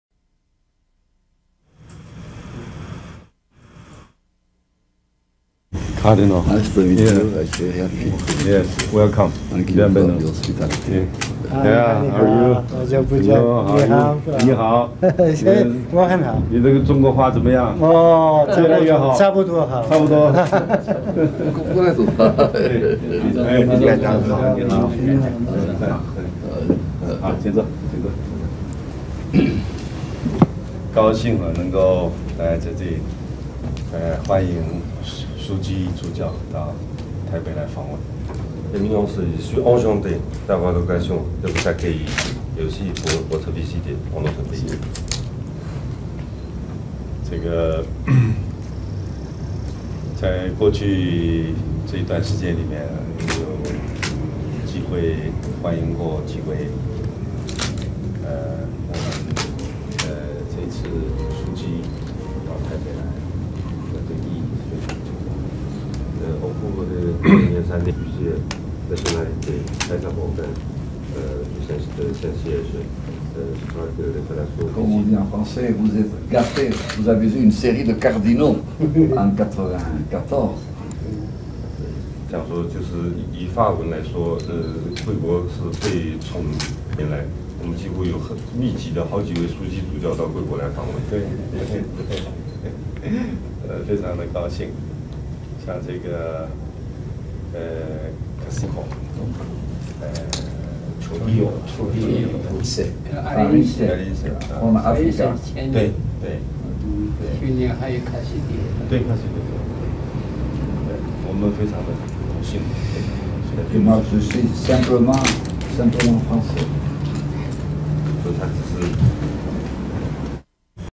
1995年01月20日連戰院長接見教廷正義暨和平委員會主席艾哲格樞機主教
1995年01月20日連戰院長接見教廷正義暨和平委員會主席艾哲格樞機主教